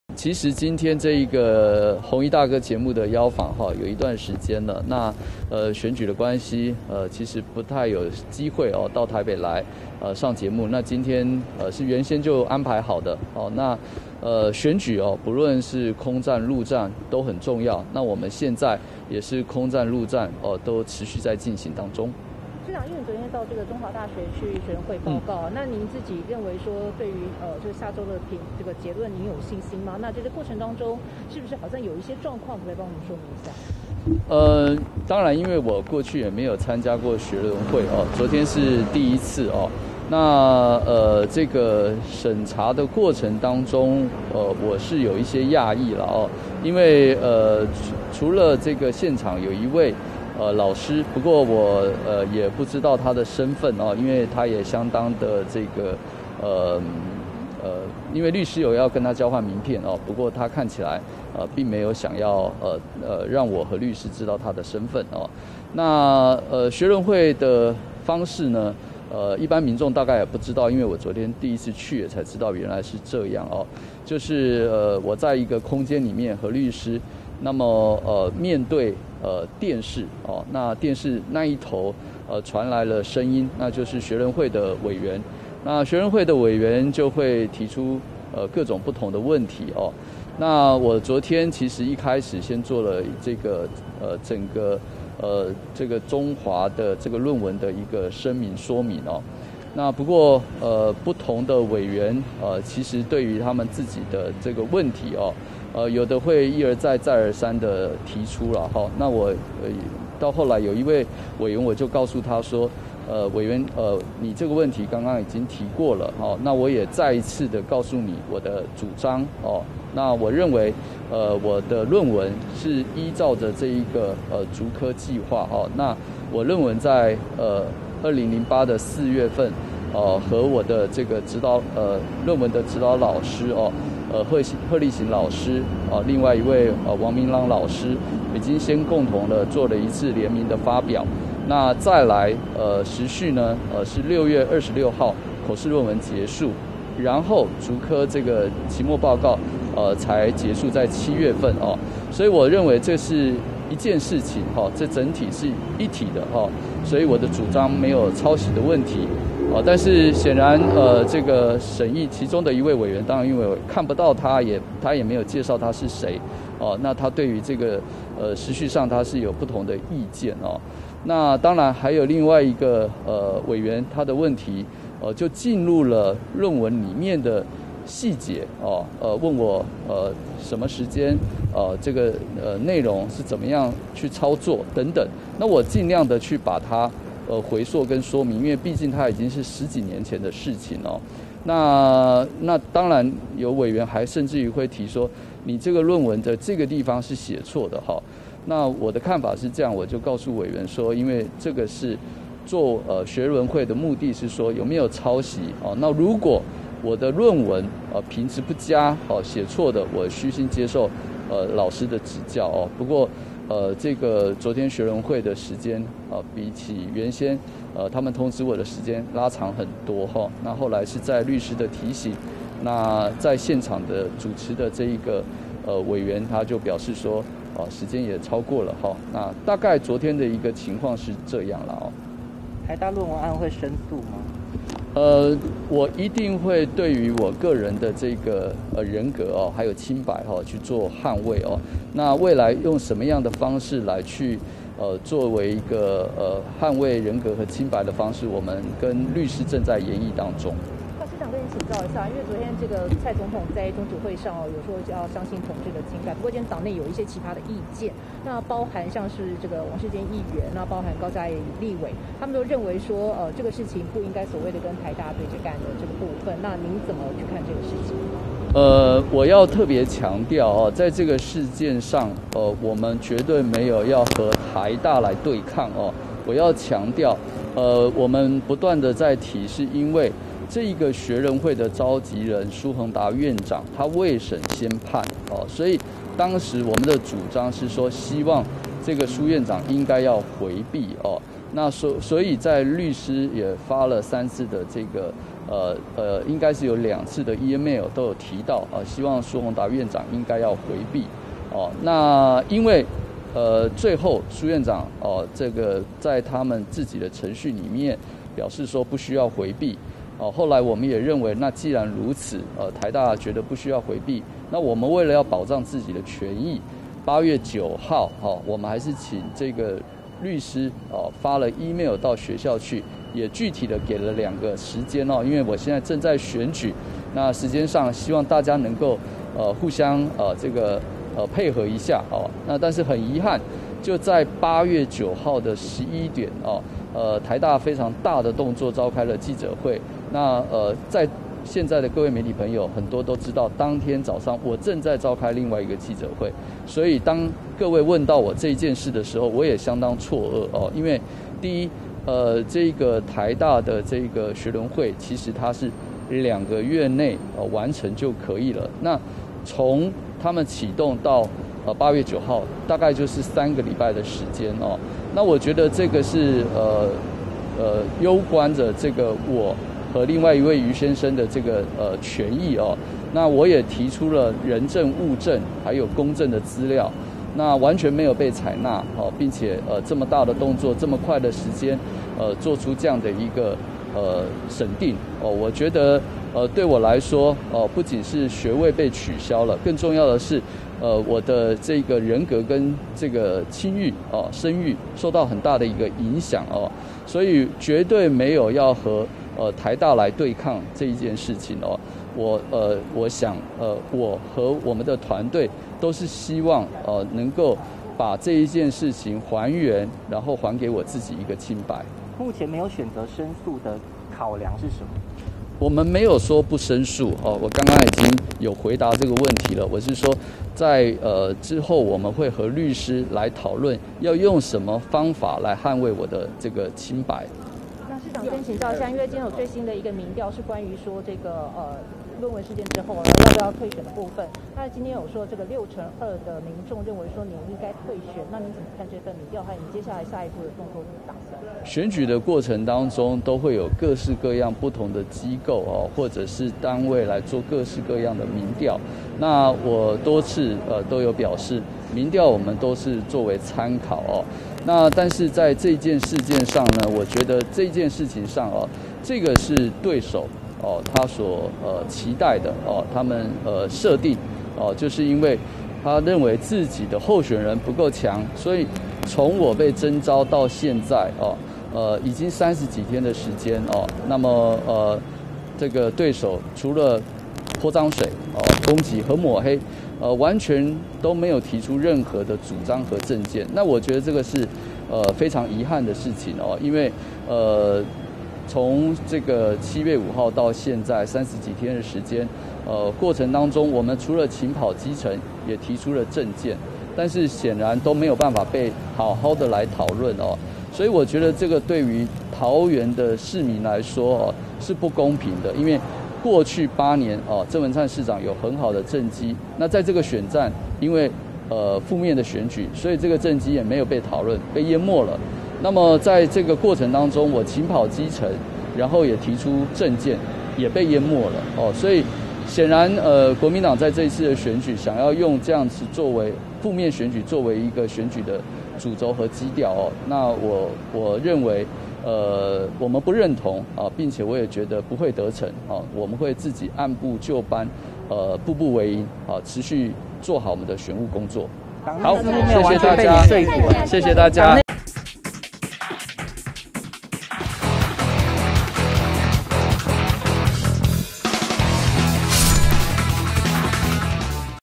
全文／林智堅親自回應！不和臺大對抗 「一定會捍衛人格和清白」│政常發揮
友邦連結區
◎ 訂閱【三立新聞網】最新影片搶先看